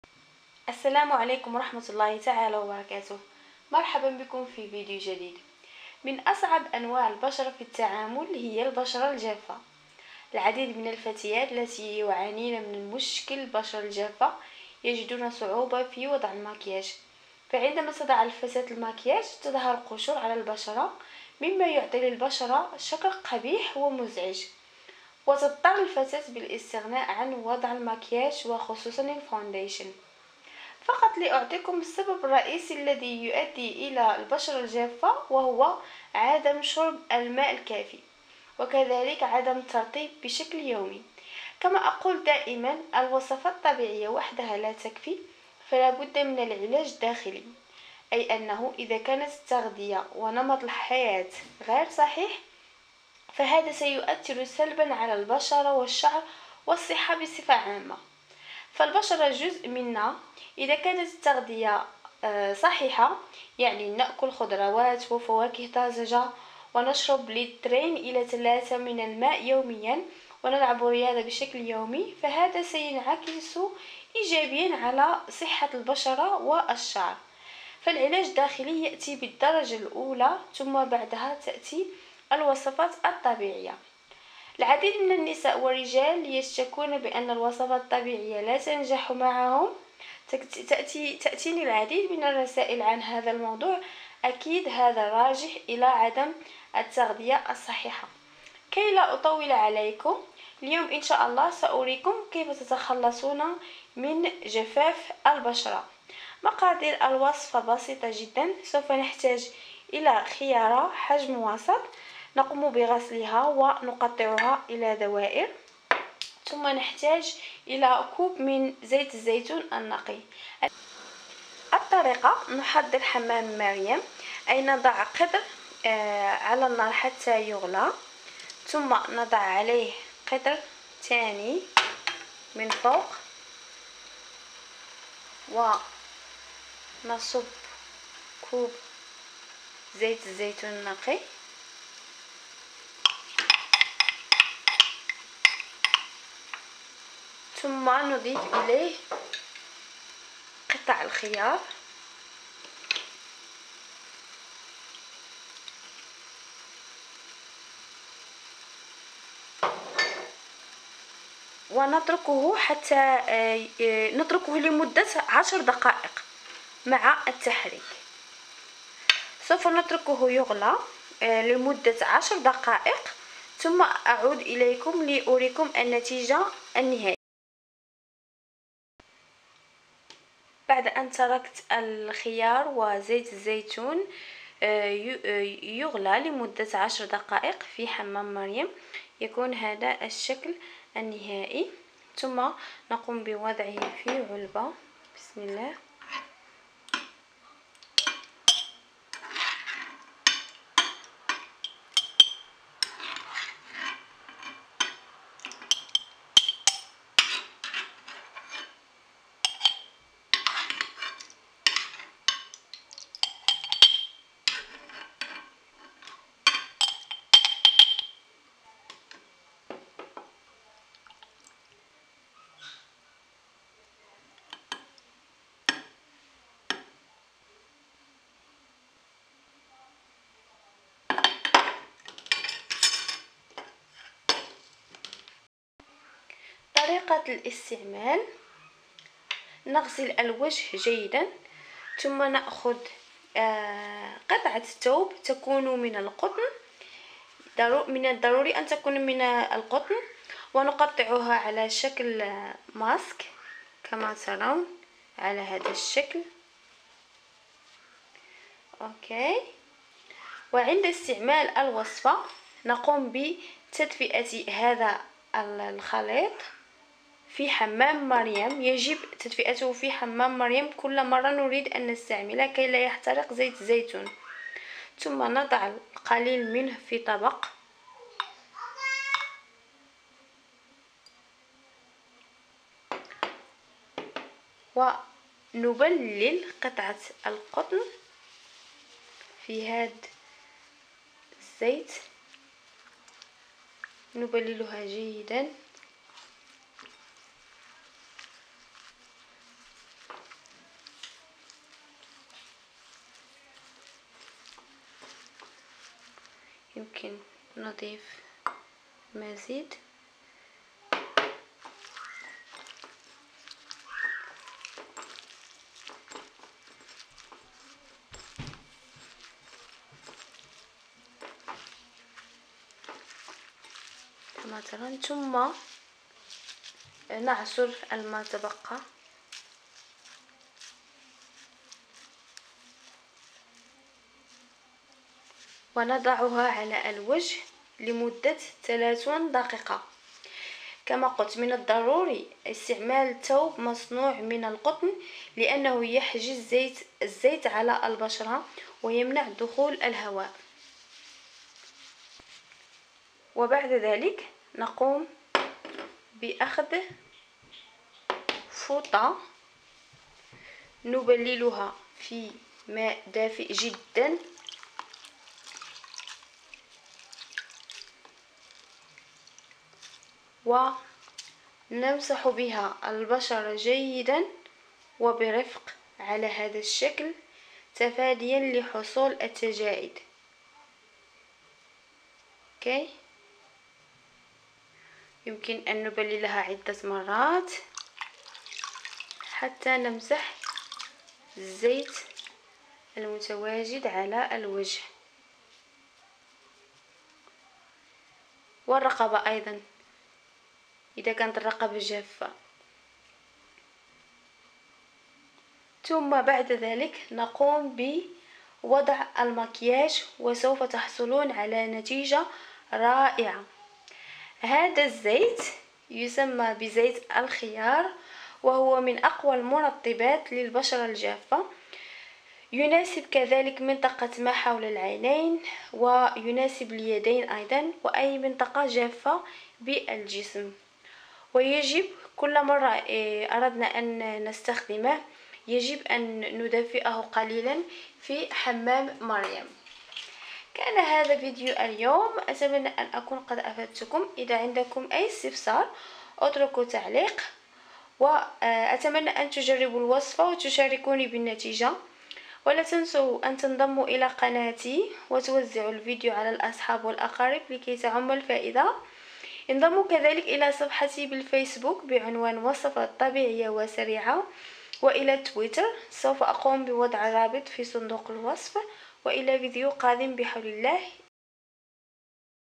علاج قوي للبشرة الجافة
المقادير
خيارة حجم وسط مقطعة إلى دوائر
كوب زيت زيتون نقي
الطريقة
نضع الخليط في حمام مريم لمدة 10 دقائق ثم نحتفض به في علبة
يجب تدفئة الزيت كل مرة نريد إستعماله في حمام مريم أو microwave
طريقة الإستعمال
نأخد قماش قطن ونقطعه على شكل ماسك ثم نغسل الوجه جيدا بالغاسول اليومي ثم نبلل القطن في الزيت ونضعه على الوجه لمدة 30 دقيقة.
بعذ ذلك نبلل فوطة في ماء دافئ ونمسح بها الوجه جيدا ثم نمسح بماء الورد باستعمال قطنة